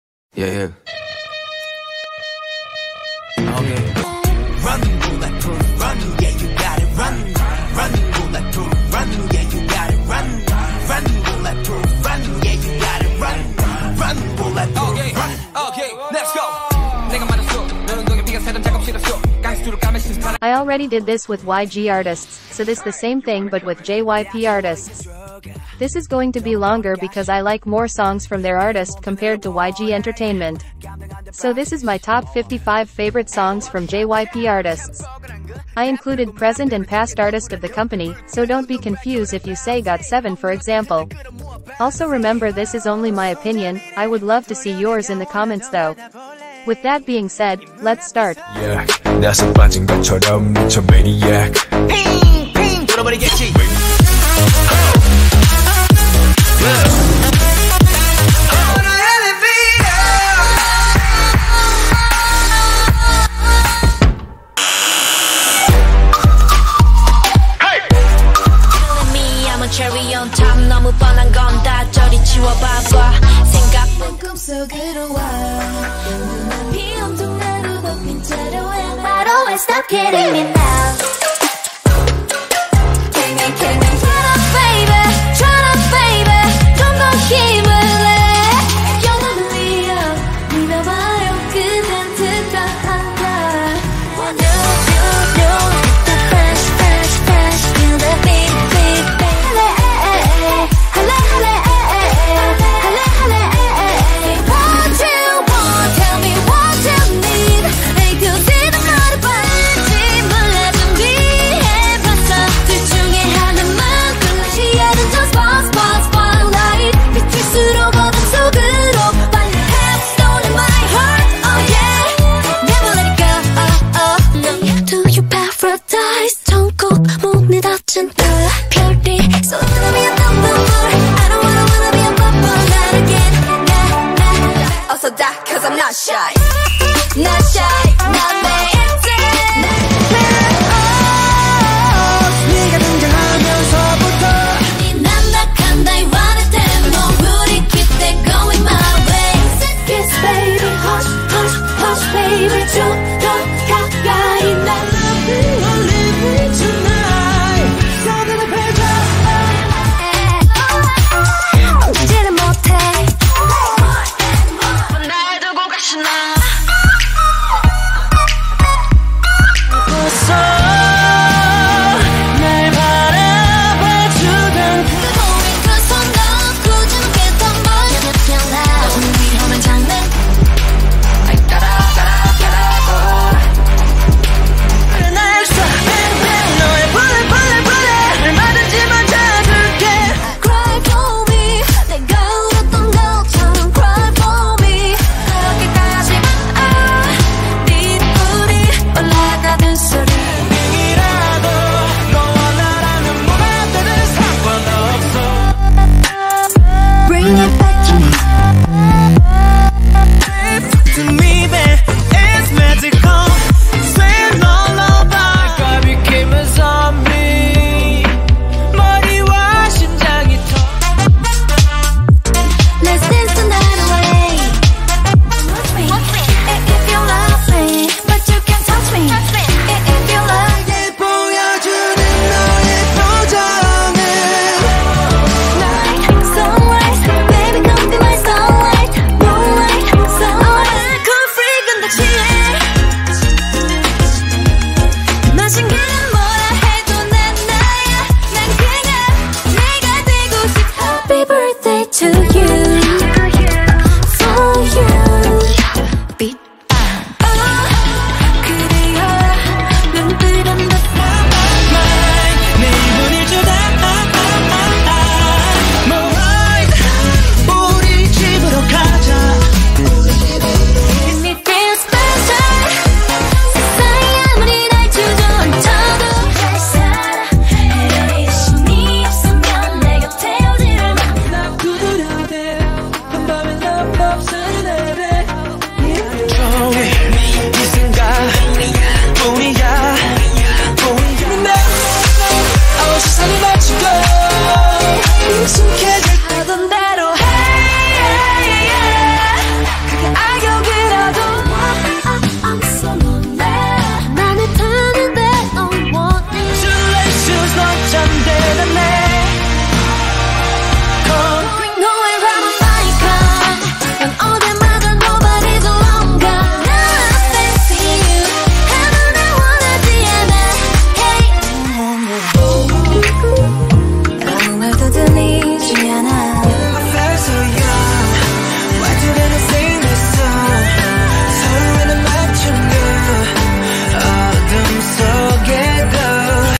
My Top 55 JYP Artists Songs
Today, I'm telling you my top 55 JYP artists songs, please respect that this is just my opinion and ranking. you can tell me yours in the comments!!